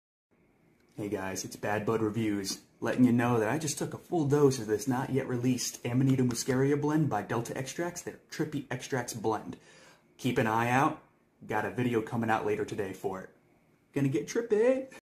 🍄Trippy Extrax🍄 Sneak Peak 😜✌️ Amanita Muscaria Complex 🧪
Delta Extrax - Trippy Extrax: Amanita Complex
• Flavor: Lemon Dream
• Suggested Use: Place drops in an incense vape or apply 1 drop to skin and rub it in.
• Ingredients: Muscimol (Amanita Muscaria), Hemp seed oil, Flavorings, Delta-9 THC (15mg), CBD, Kava & Lions Mane.

Thank you all for any bit of support - continued or new :)

💚Found at the ‘I Heart CBD’ shops in Charleston, SC! 💚

Use “BAD15” at checkout to save 15%!